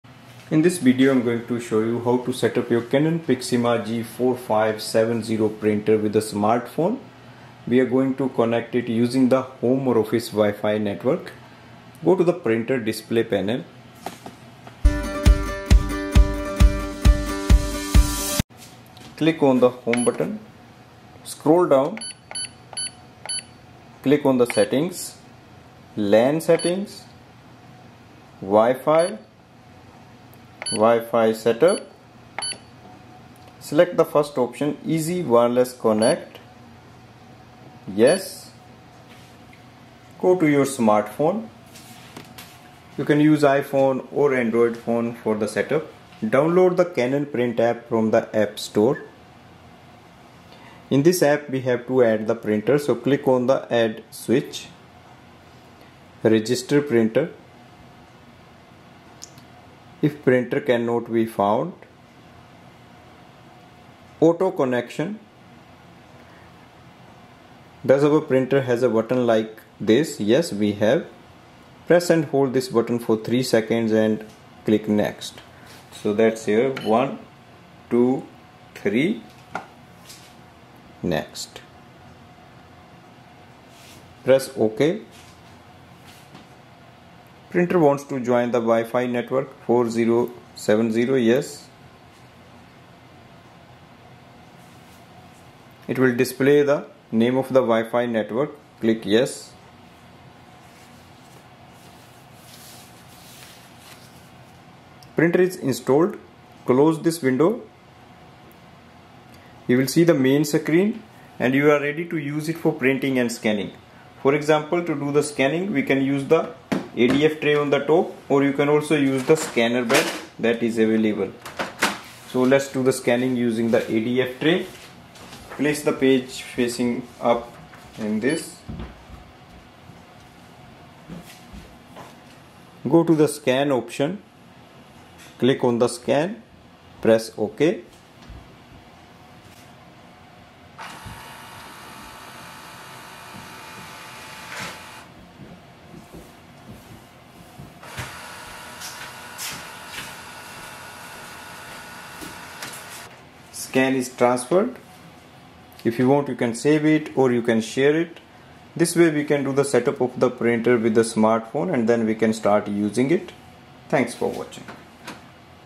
Canon Pixma G4570 Printer Setup For Phone Using Easy Wireless Setup Method !
This video reviews how to connect your Canon Pixma G4570 All-in-one printer to a smartphone using the easy wireless setup method.